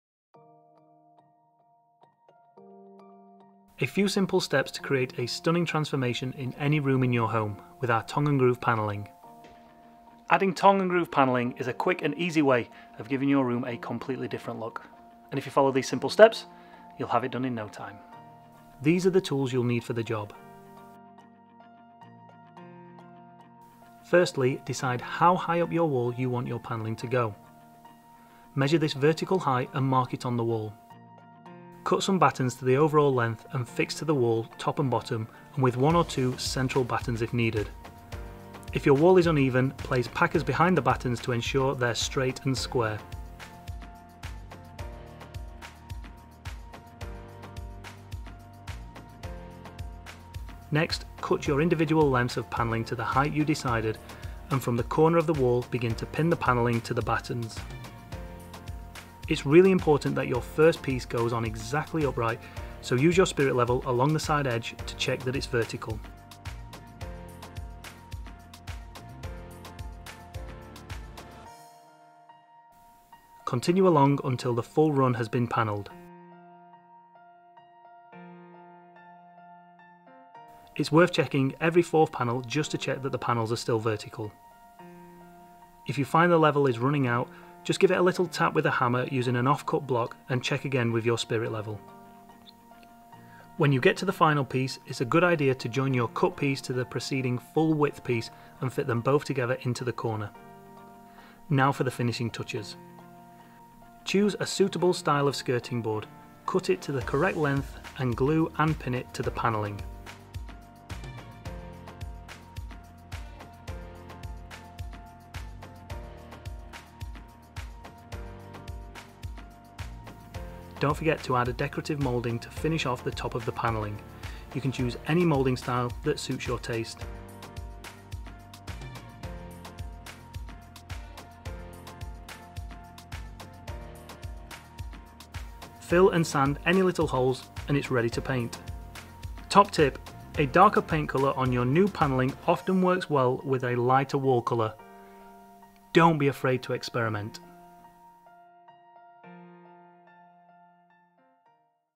How to install Tongue and Groove Panelling
Installation video shows you how to install Tongue and Groove Wall Panelling. Download full fitting instructions here to compliment the video